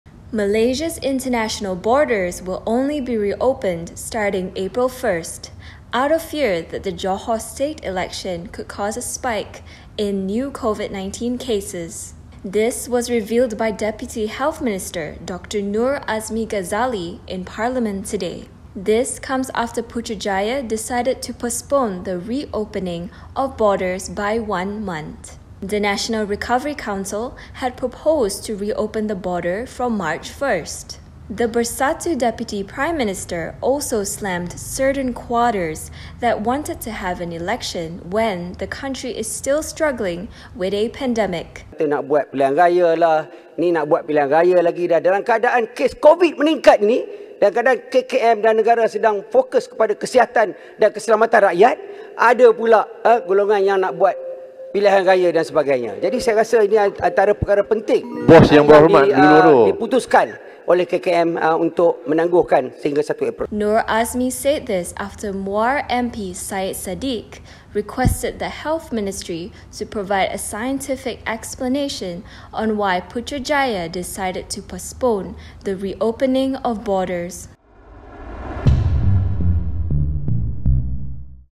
Border reopening delayed in fear of Covid spike after Johor election
The decision to postpone the reopening of Malaysia's international borders from March 1 to April 1 was made out of fear that the Johor election could have caused a spike in new Covid-19 cases, said Deputy Health Minister Dr Noor Azmi Ghazali.

In answering a question from Syed Saddiq Syed Abdul Rahman (Muda-Muar), the Bersatu minister also slammed "certain quarters" that wanted to have an election when the country was still struggling with a pandemic.